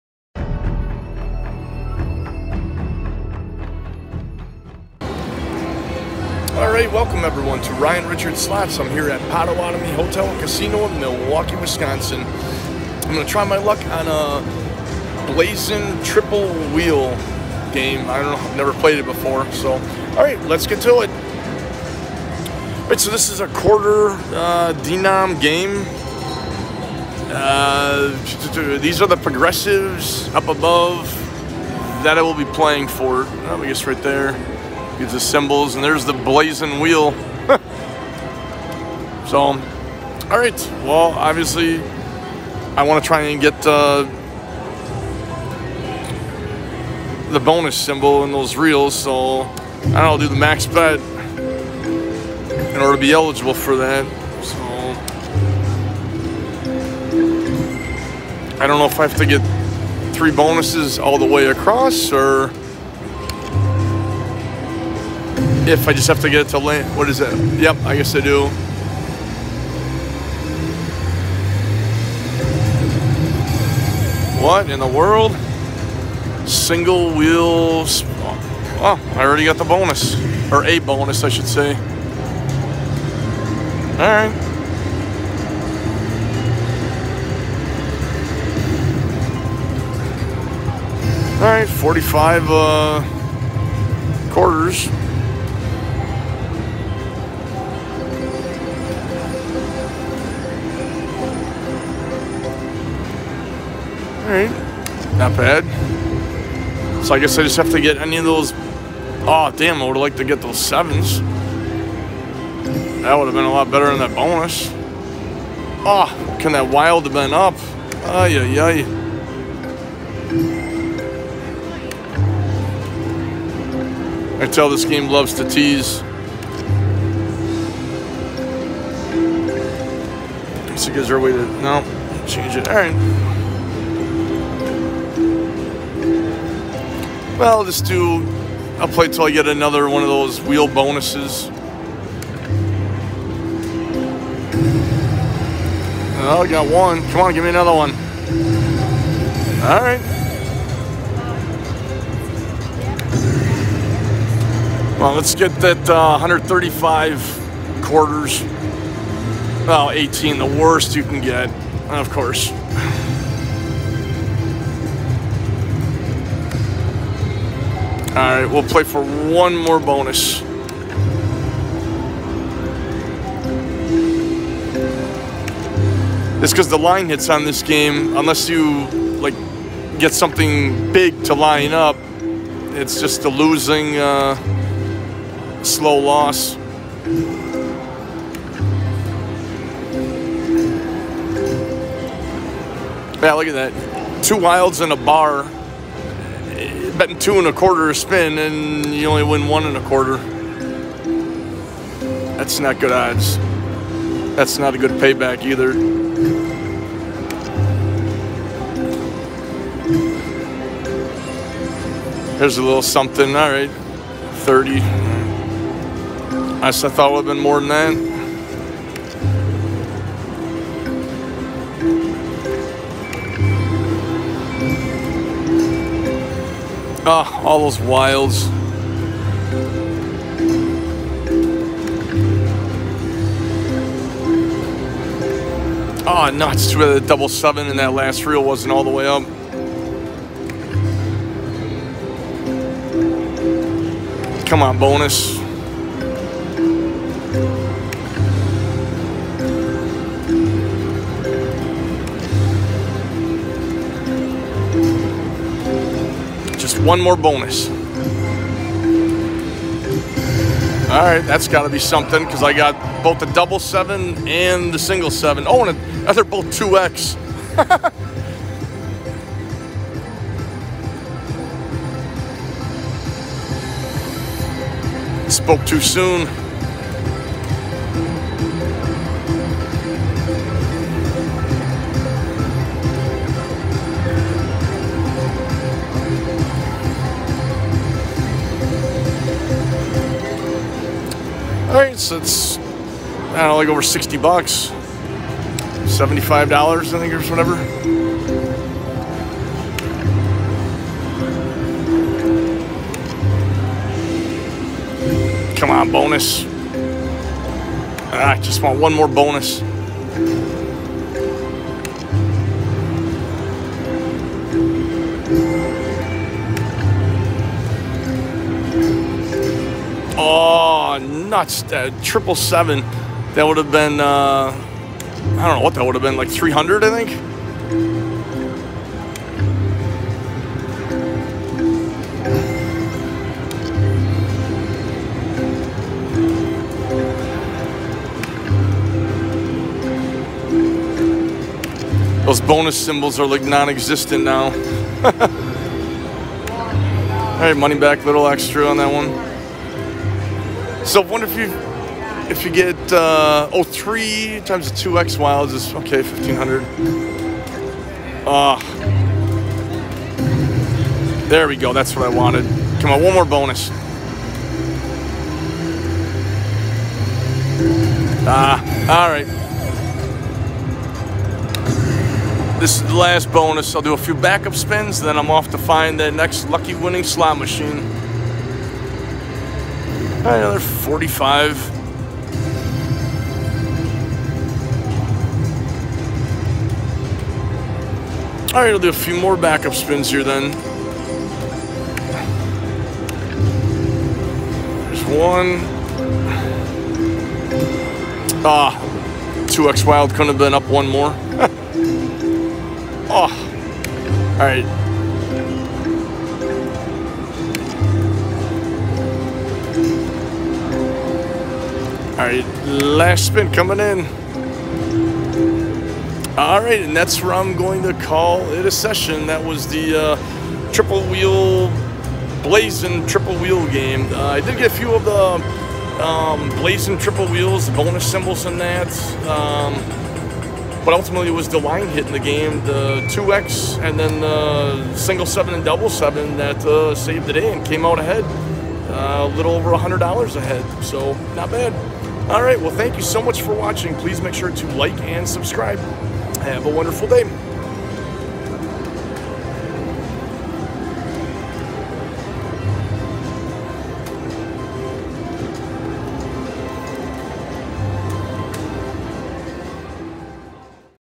Ryan Richard Slots "Triple Blazing 7's" Slot Machine at Potawatomi Hotel & Casino
w/a few wheel spin feature bonus spins